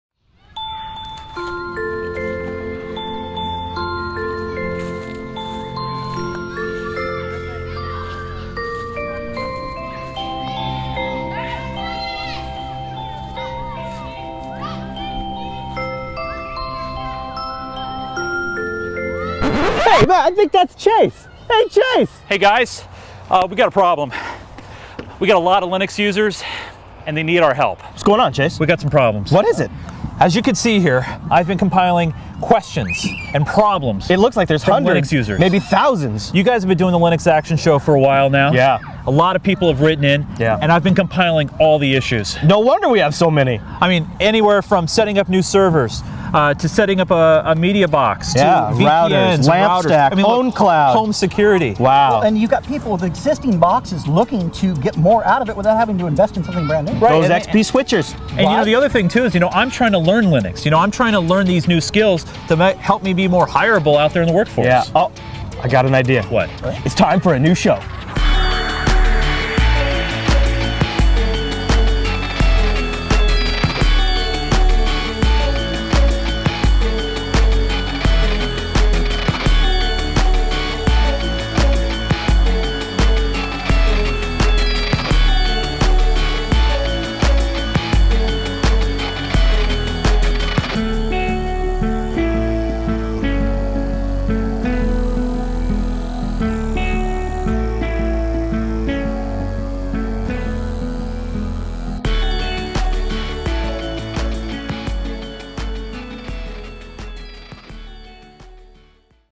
NEW SHOW: Howto Linux - COMING SOON!
Jupiter Broadcasting is happy to announce Howto Linux. A new weekly podcast coming soon.

Want to be our producer? Fill out our application